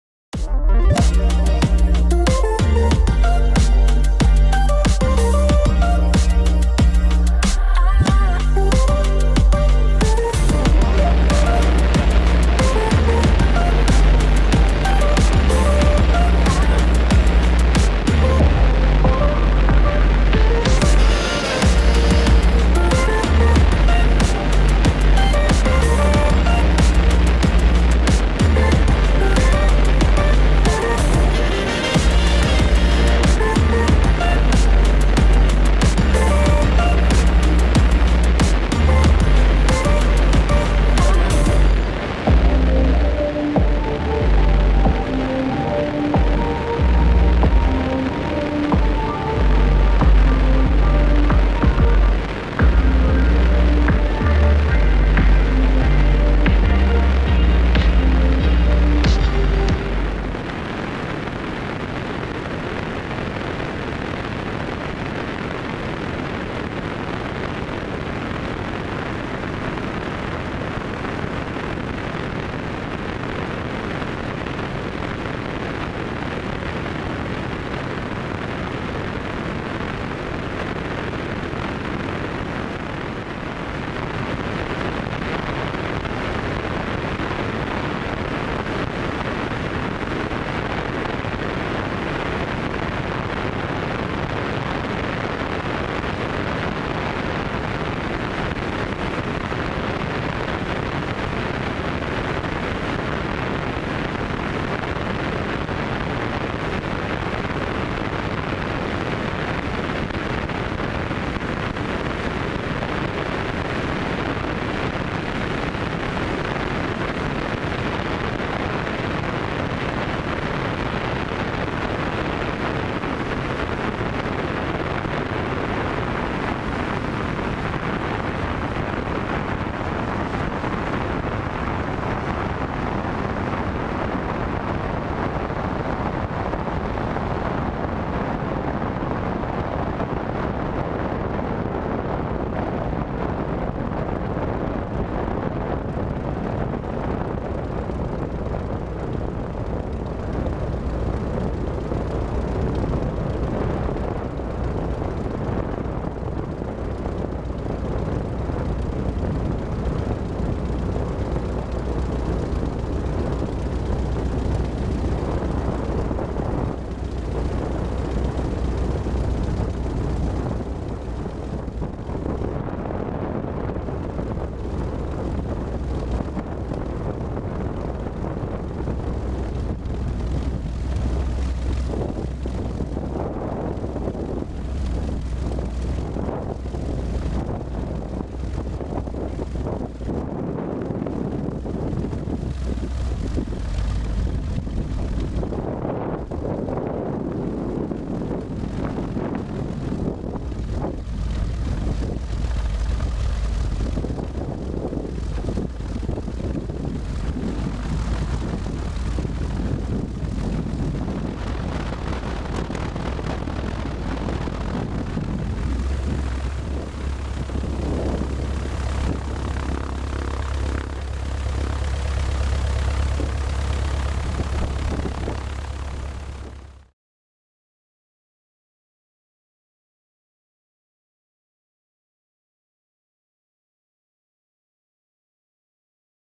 Zenith Test Flight After In - Flight Alternator Failure!! How will it Go?
Test flight on a beautiful (still) summer day, after a recent in-flight alternator failure to this same airport!   Filmed with the Go Pro Hero 3+.